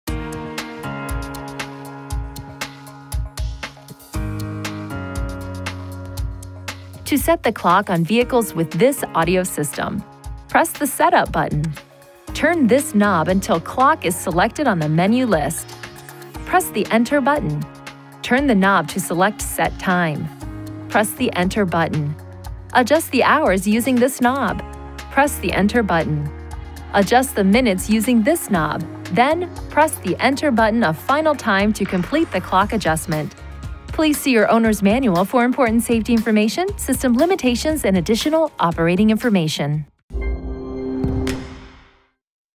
2017 NISSAN Sentra - Setting the Clock without Navigation Type B (if so equipped)
To set the clock on vehicles with THIS audio system:
● Press the SETUP button,
● Turn THIS knob until CLOCK is selected on the menu list,
● Press the ENTER button,
● Turn the knob to select SET TIME,
● Adjust the hours using THIS knob,
● Adjust the minutes using THIS knob, then
● Press the ENTER button a final time to complete the clock adjustment.